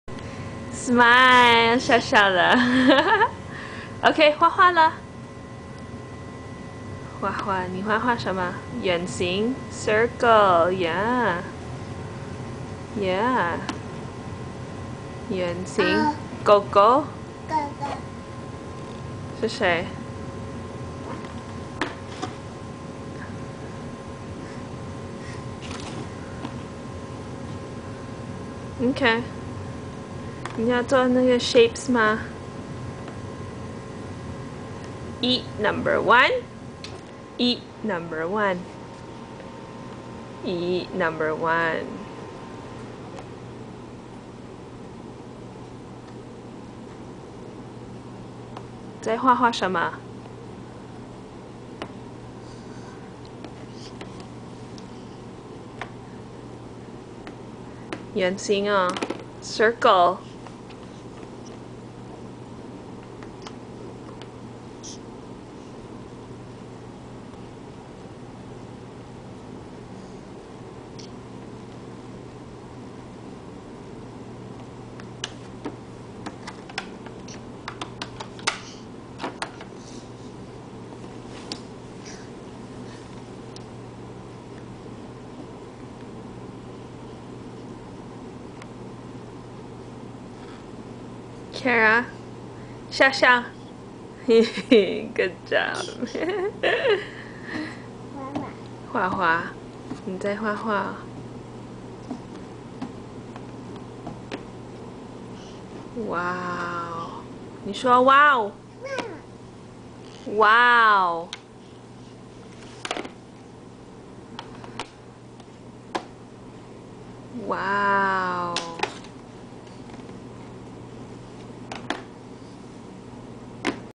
Cara drawing on her doodle board
Cara loves her new doodle board.